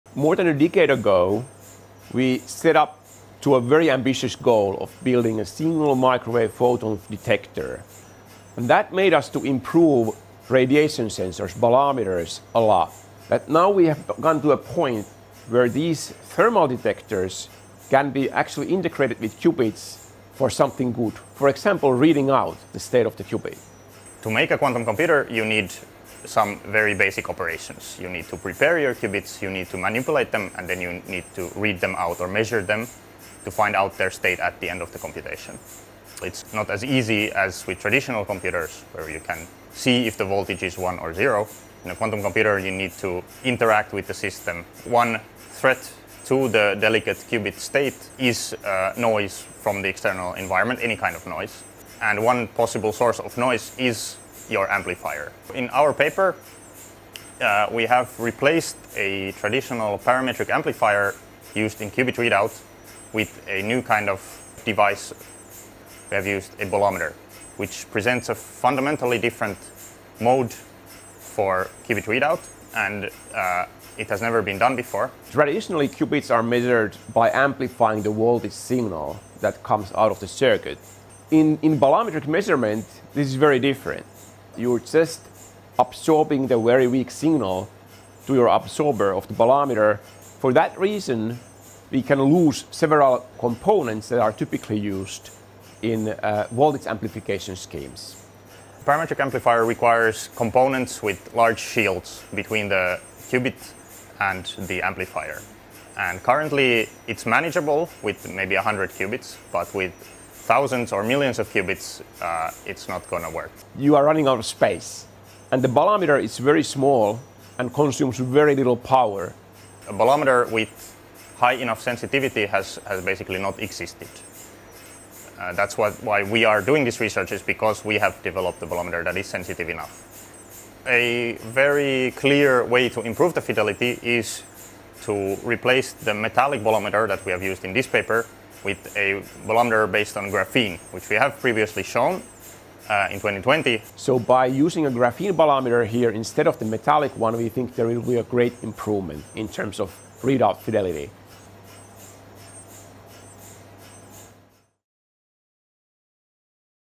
Nanobolometers promise ease of scalability in microscopic package
Link to research paper:
András M. Gunyhó, Suman Kundu, Jian Ma, Wei Liu, Sakari Niemelä, Giacomo Catto, Vasilii Vadimov, Visa Vesterinen, Priyank Singh, Qiming Chen, Mikko Möttönen, Single-Shot Readout of a Superconducting Qubit Using a Thermal Detector, Nature Electronics, (2024).